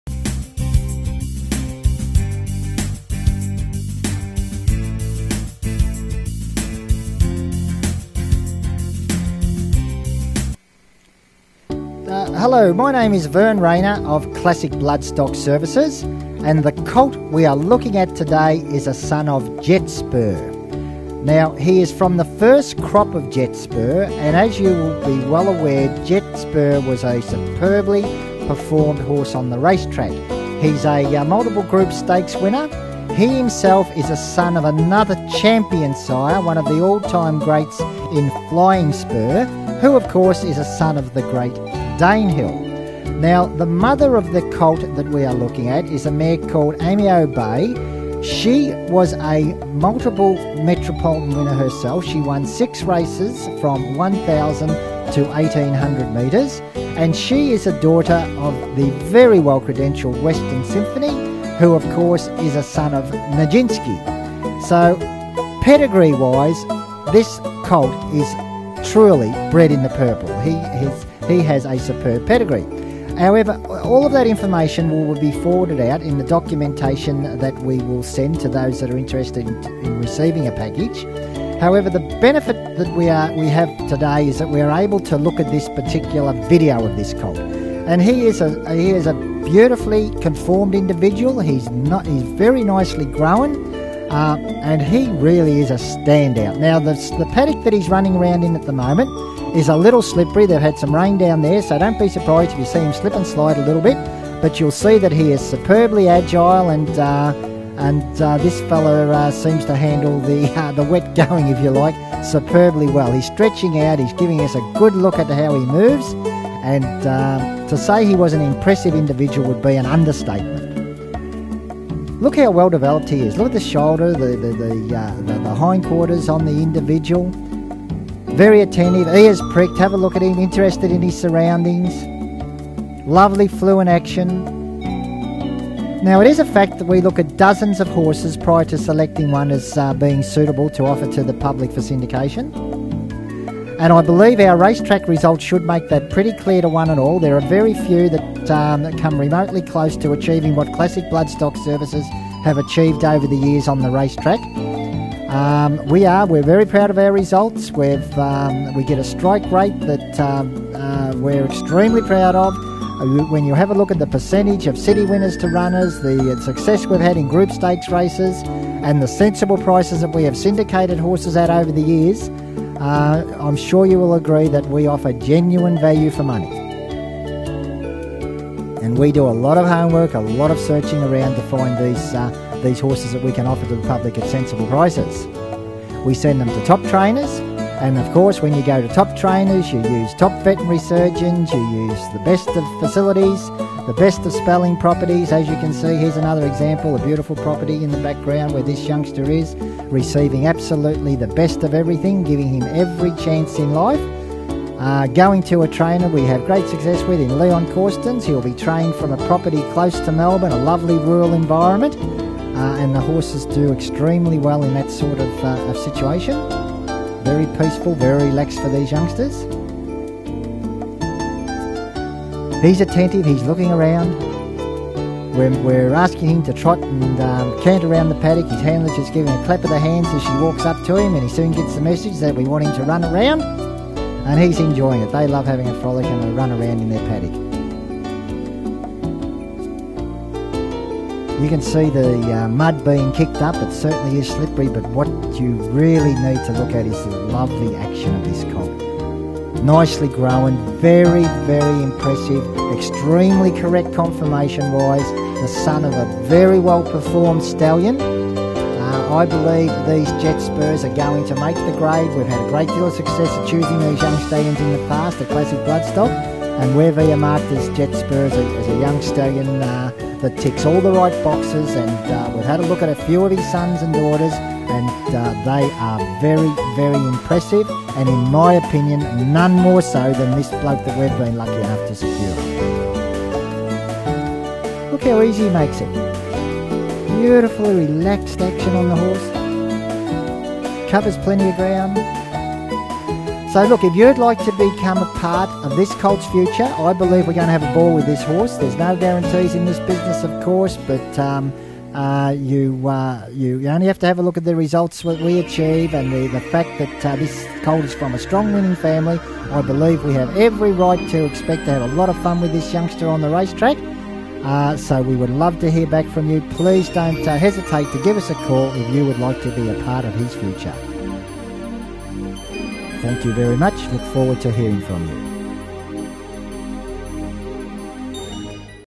Racehorse Colt by Jet Spur
Racehorse Colt by Jet Spur. Syndicate shares available from Classic Bloodstock Services.

A SON OF JET SPUR

A Dual Group Stakes Winning Son of the Legendary

FLYING SPUR

There is always an element of excitement when offering the progeny of a first season sire, as we all know that if we get it right, the rewards can be enormous, as sons and daughters of young stallions that fire with their first crop become virtually impossible to buy the following year at the yearling sales, as their value goes through the roof.

Jet Spur is a stallion with all of the attributes we look for in a sire. As a racehorse he was right up there with the best of them, he won at the highest level (Group Stakes level). He is superbly bred, being a son of one of Australias all time great sires in Flying Spur (Champion Australian Sire), himself a son of the immortal Danehill.

The dam of Jet Spur was also a Group Stakes winner, and is herself a daughter of another Superstar sire in Bletchingly (Champion Australian sire 3 times).

Eimeo Bay, the dam of the colt on offer, is a multiple city winning mare of 6 races in all from 1000m to 1800m and is a daughter Western Symphony (by Nijinsky) which of course gives the colt on offer a DOUBLE CROSS of the great Northern Dancer.